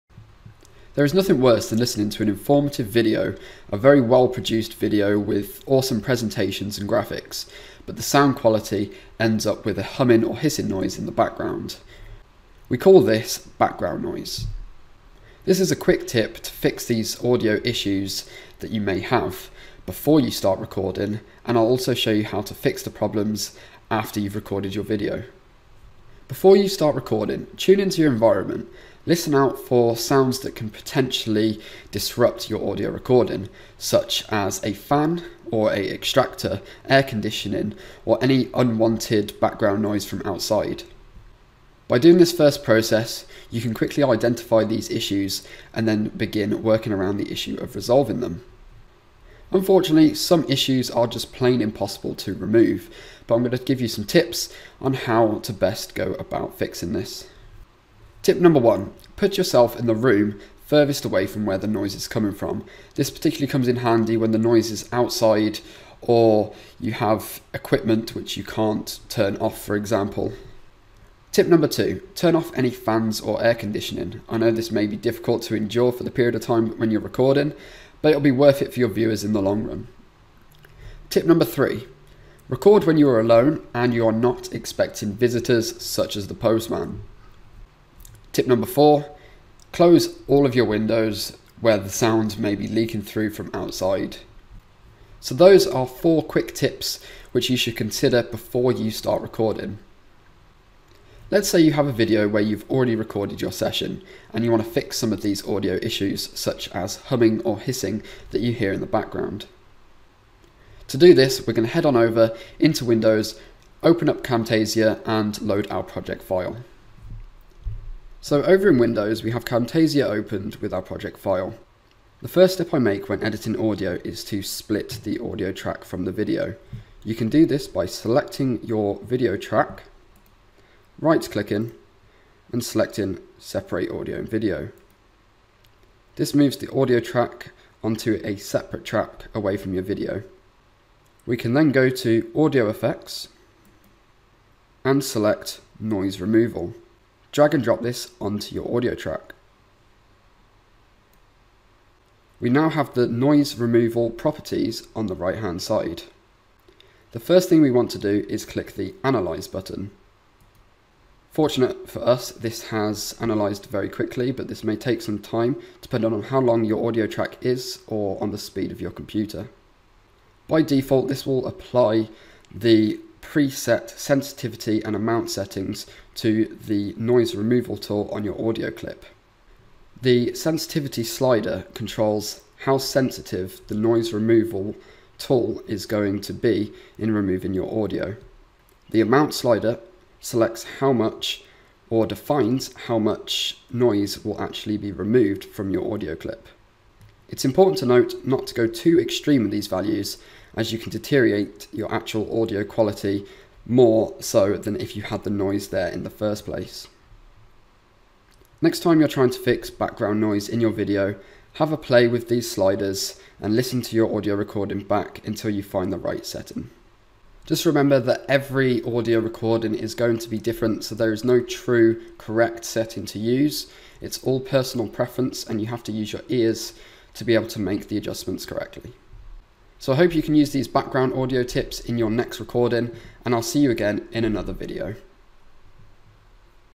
A simple way to reduce or remove Background Noise
I will share with you some simple tips and tricks to help you reduce background noise in your videos.

These are some simple tips, but can be very much overlooked when starting out producing videos.

You'll learn some tricks to help reduce background noise before you start recording and also remove background noise from pre-recorded videos in post using Camtasia